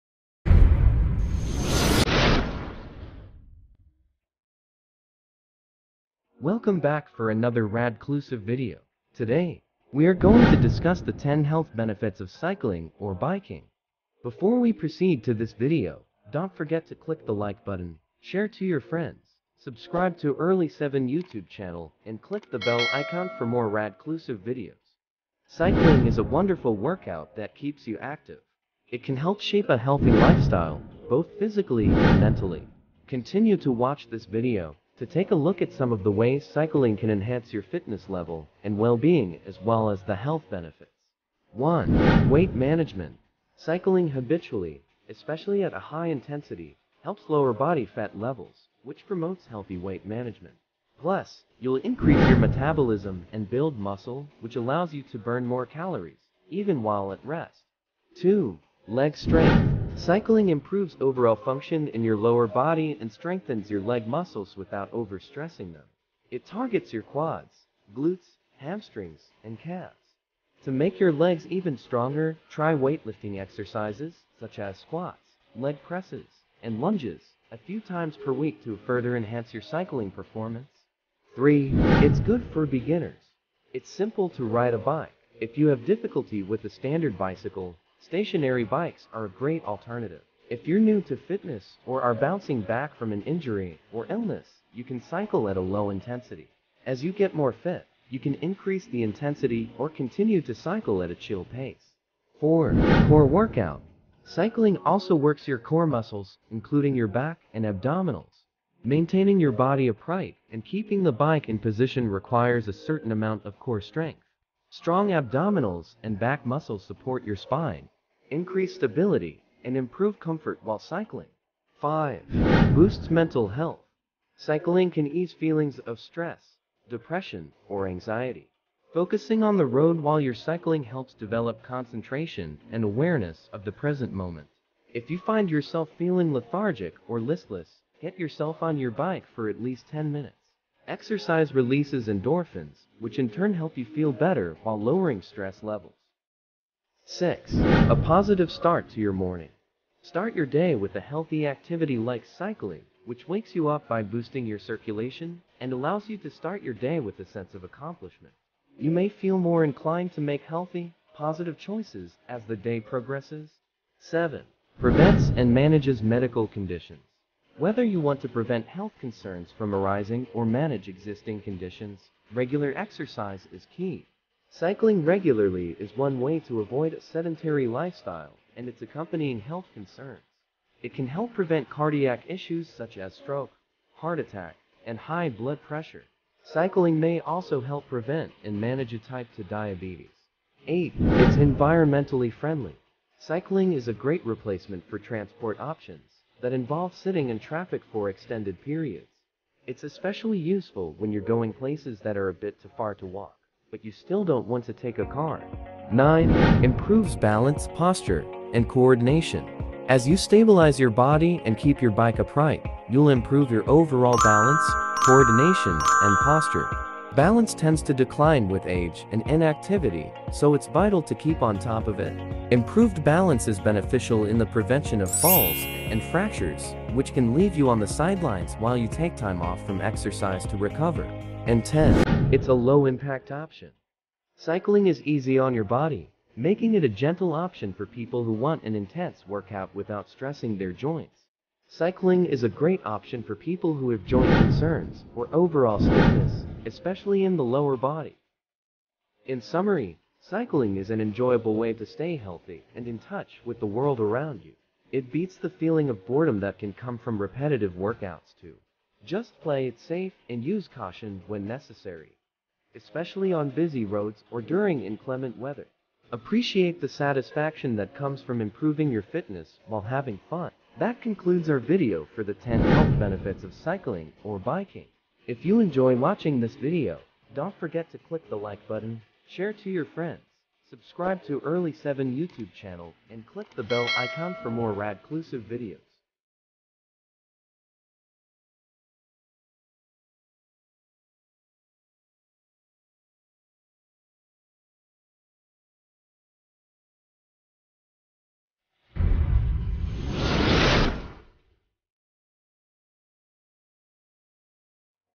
HOW TO BE HEALTHY? || TOP 10 HEALTH BENEFITS OF BIKING || CYCLING HEALTH BENEFITS
10 HEALTH BENEFITS OF CYCLING OR BIKING

Cycling or biking is a low impact aerobic exercise that offers a wealth of benefits. It also varies in intensity, making it suitable for all levels. You can cycle as a mode of transport, for casual activity, or as an intense, competitive endeavor.

Cycling is a wonderful workout that keeps you active. It can help shape a healthy lifestyle, both physically and mentally.

Continue reading to take a look at some of the ways cycling can enhance your fitness level and well-being.

Cycling for health and fitness

It only takes two to four hours a week to achieve a general improvement to your health. Cycling is:

Low impact – it causes less strain and injuries than most other forms of exercise.

A good muscle workout– cycling uses all of the major muscle groups as you pedal.

Easy – unlike some other sports, cycling does not require high levels of physical skill. Most people know how to ride a bike and, once you learn, you don’t forget.

Good for strength and stamina– cycling increases stamina, strength and aerobic fitness.

As intense as you want– cycling can be done at very low intensity to begin with, if recovering from injury or illness, but can be built up to a demanding physical workout.

A fun way to get fit– the adventure and buzz you get from coasting down hills and being outdoors means you are more likely to continue to cycle regularly, compared to other physical activities that keep you indoors or require special times or places.

Time-efficient – as a mode of transport, cycling replaces sedentary (sitting) time spent driving motor vehicles or using trams, trains or buses with healthy exercise.